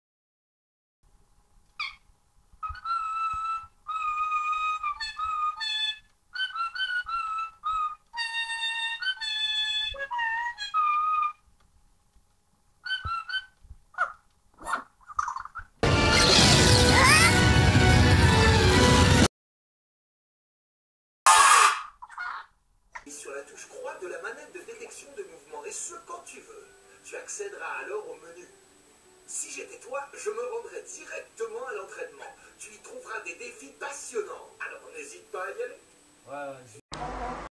Hunchrooster Adventure Clip: Pups's Nightmare
Pups gets a nightmare: Pups makes a song and sleep and Kirby eats Pups and Pups wake up after of the nightmare.